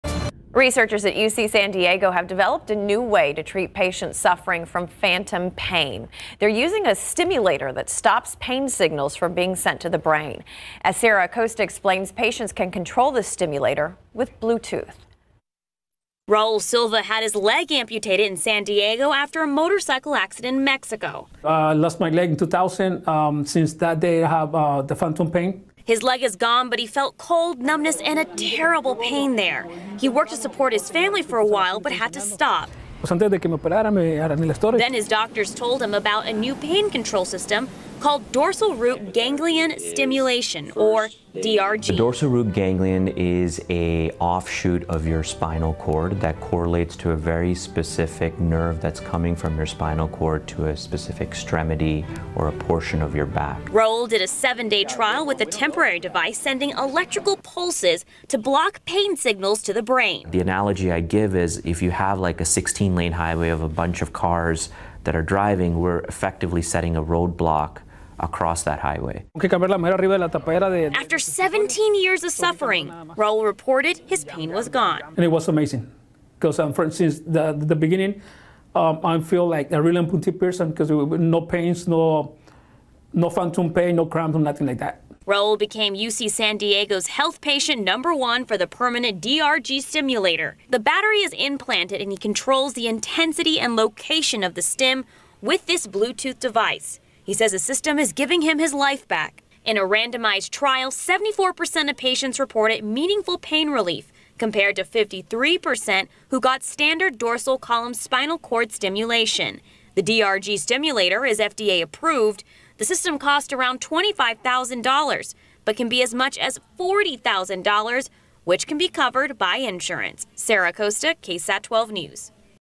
Bluetooth DRG pain control
Researchers at University of California San Diego are coming to the rescue of people with terrible pain in one part of their body.